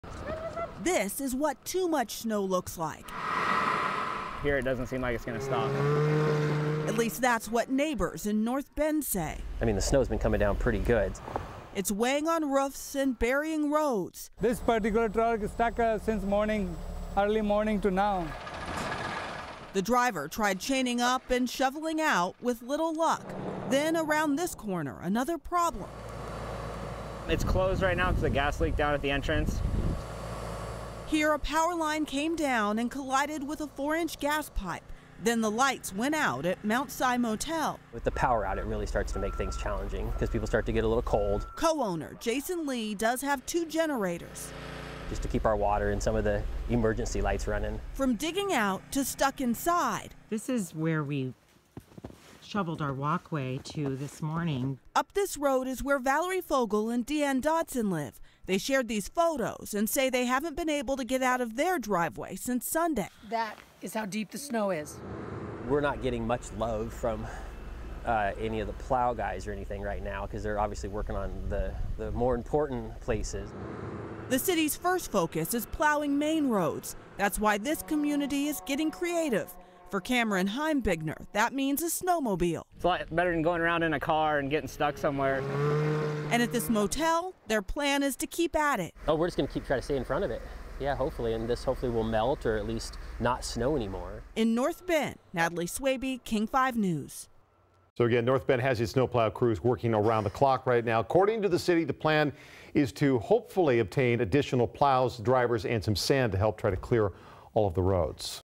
State of emergency in North Bend
North Bend declared a state of emergency because of severe weather. The city is calling in extra resources to help them handle all the snow as crews work to clear the streets.